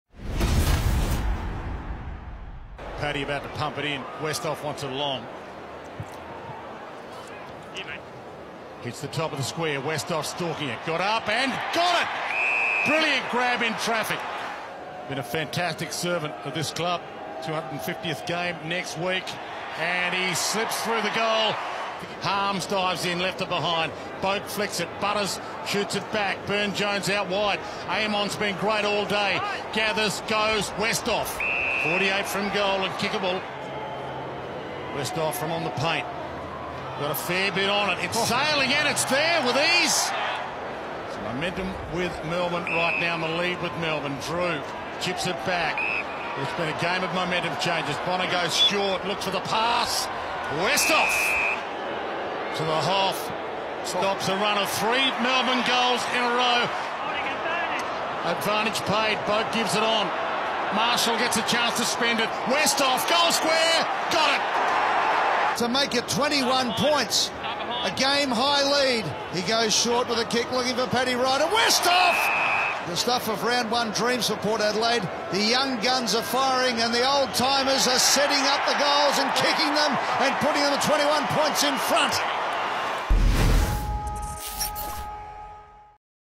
High five for the Hoff
Power utility Justin Westhoff started off his season with a bag of five goals in his side's memorable victory over the Dees